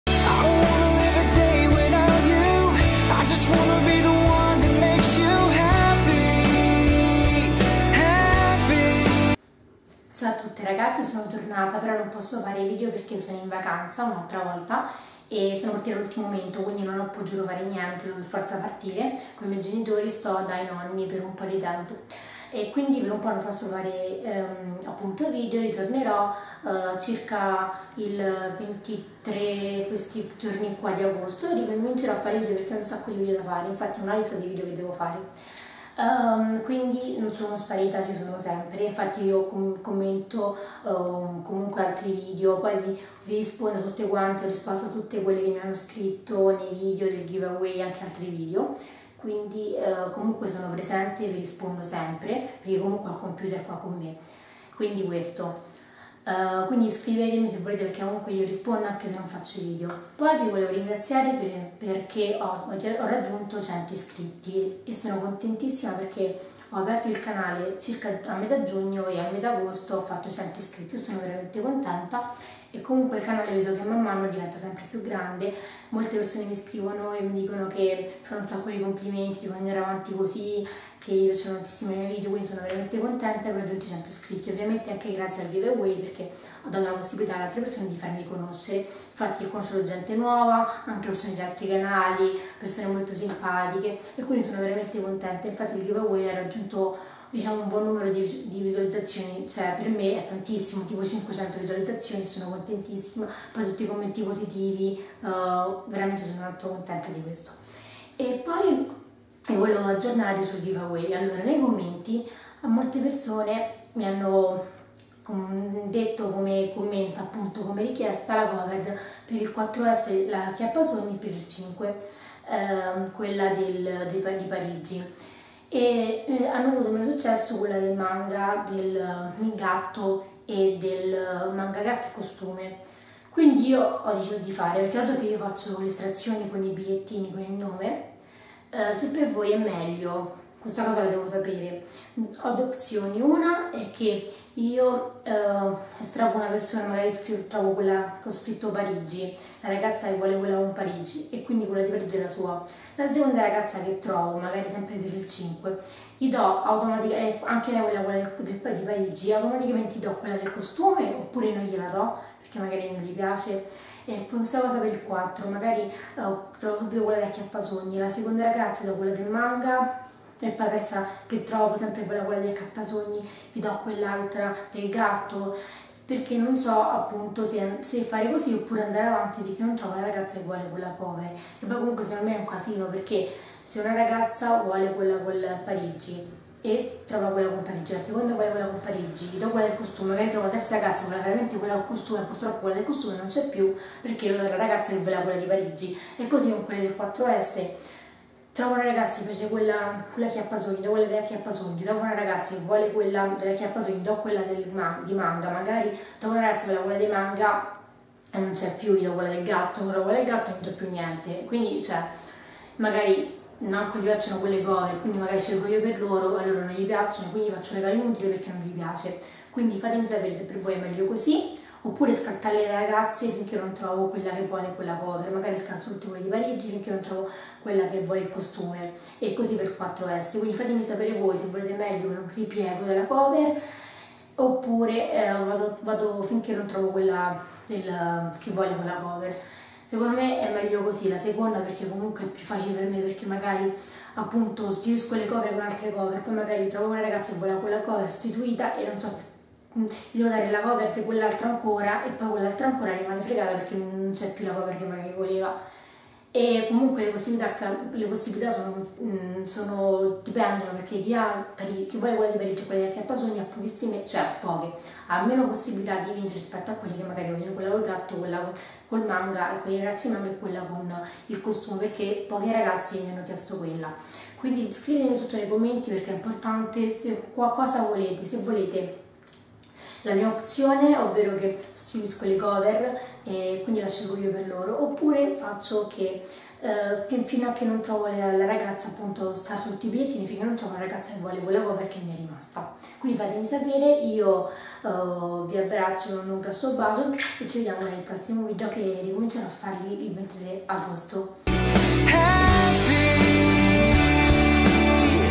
VLOG:Sono Sparita?100 ISCRITTI GRAZIE DI CUORE! Aggiornamenti Giveaway
Ragazze 1-SCUSATE IL MIO STATO UMANO SENZA TRUCCO E PRATICAMENTE USTIONATA DAL SOLE 2-Questo è un tentativo e dico tentativo di vlog,dove chiacchierò un po' con voi e vi ringrazio davvero di cuore!!è bellissimo leggere i vostri commenti!!Spero che continuiate a seguirmi!! Per quanto riguarda il giveaway ci sono delle novità quindi commentatemi sotto cosa ne pensate!!